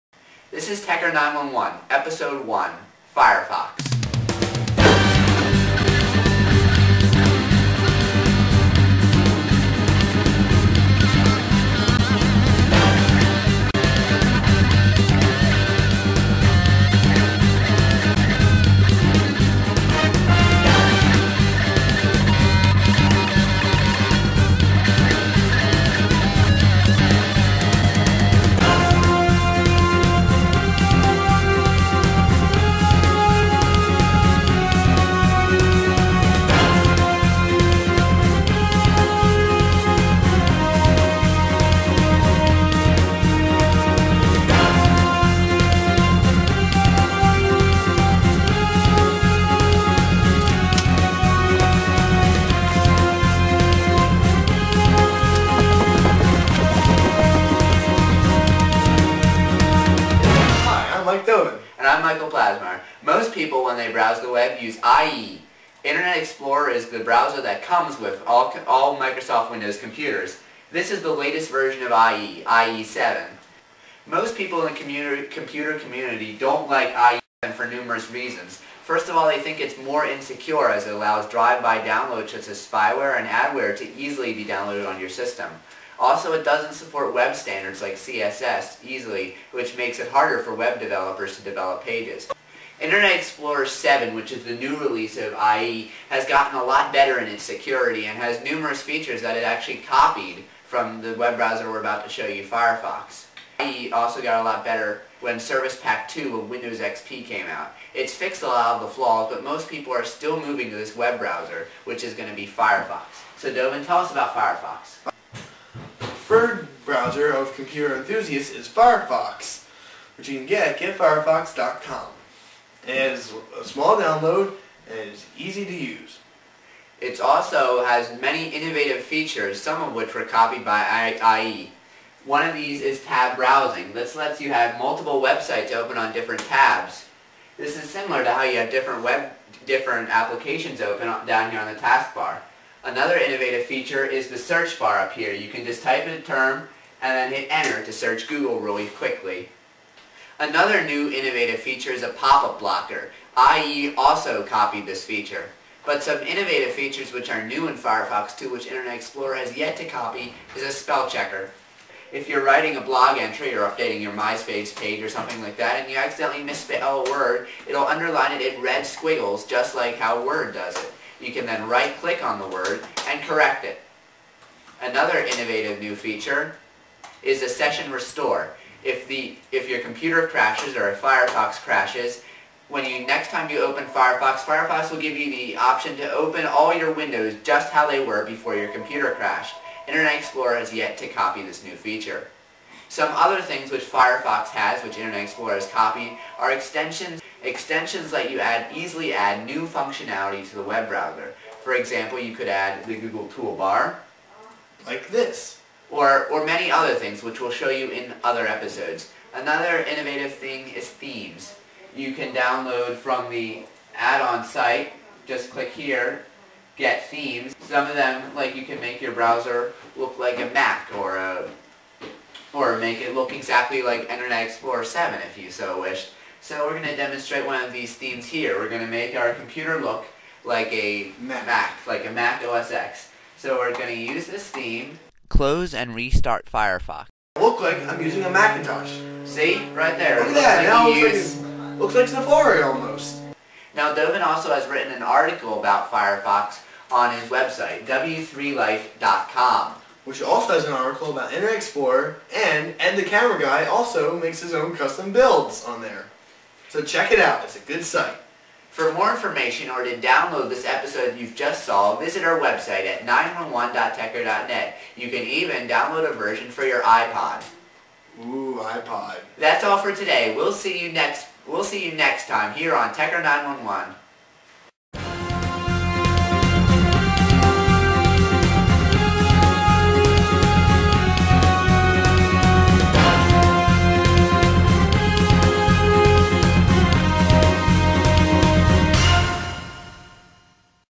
Tecker 911: Episode 1: Firefox
Michael Plasmeier and Michael Gdovin explain the browser of choice for computer enthusiasts, Firefox.